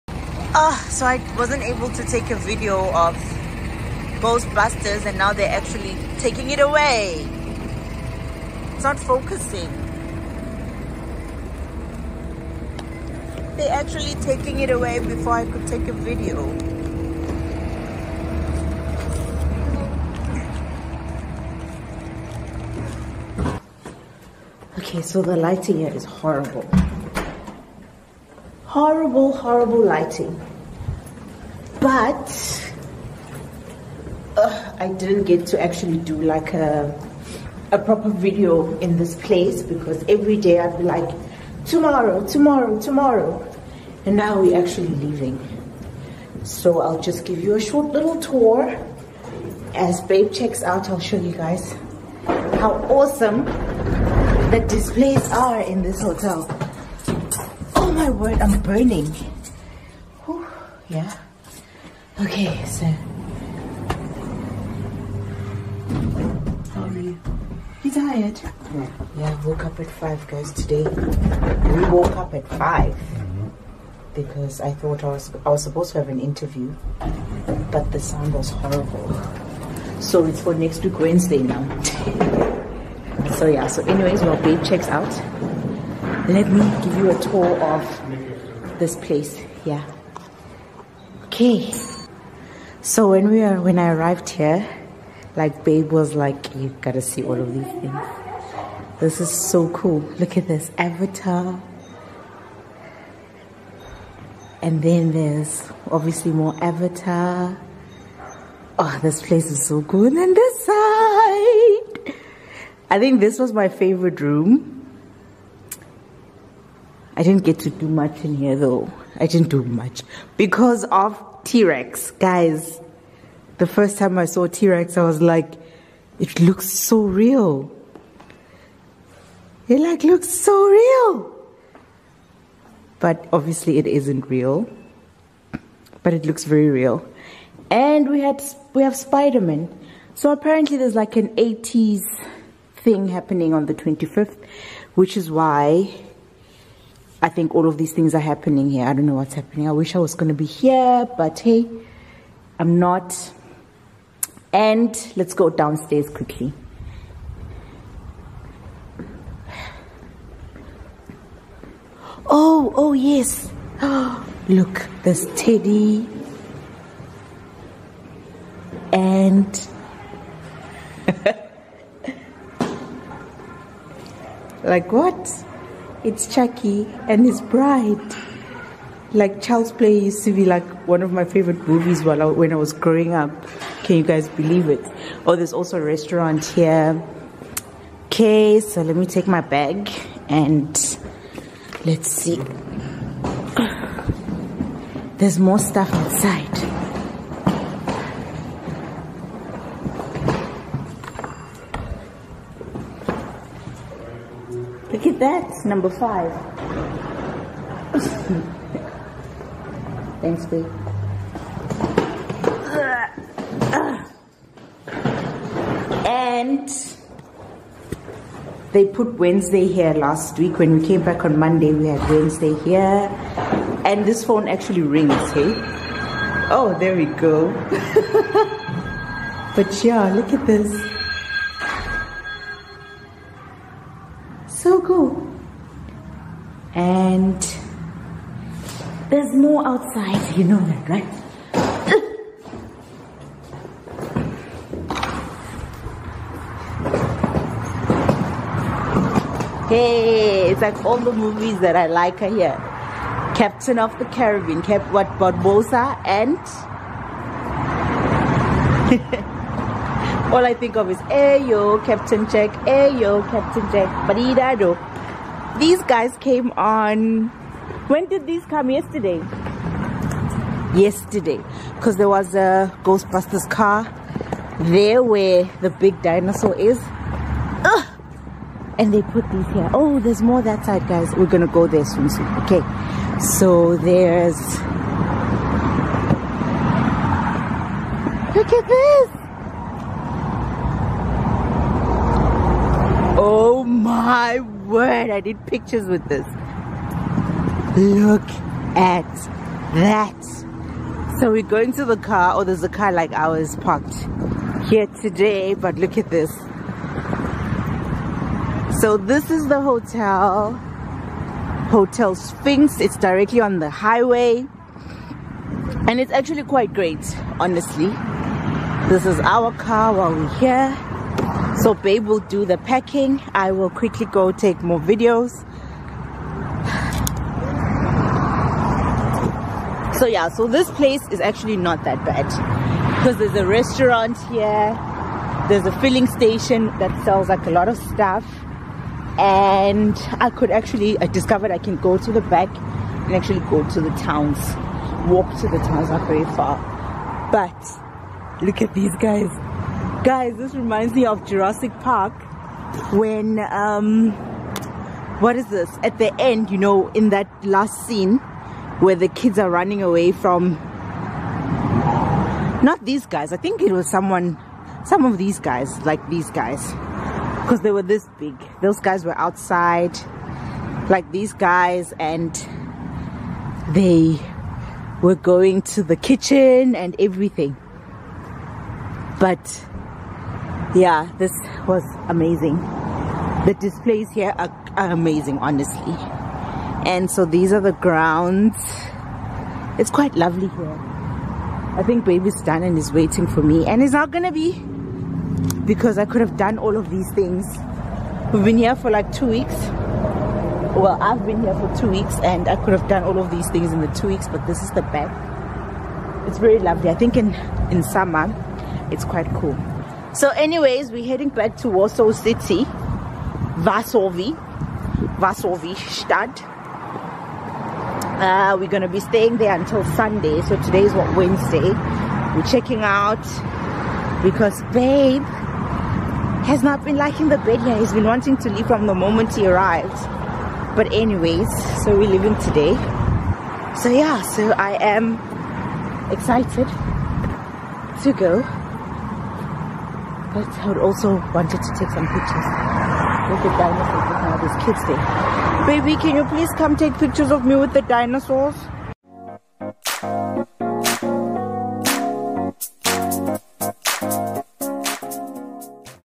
POLAND VLOG (Part 2) | Touring our movie themed hotel
Welcome to Part 2 of my Poland vlog series! In this episode, join me as I take you on a tour of our unique movie-themed hotel in Warsaw. From cinematic decor to themed rooms, this hotel offers a truly immersive experience for film lovers. Watch as I explore the quirky and fun details that make this stay unforgettable. Let's dive into this movie-themed hotel together!

“If you enjoy this video, be sure to share it and drop your comments.”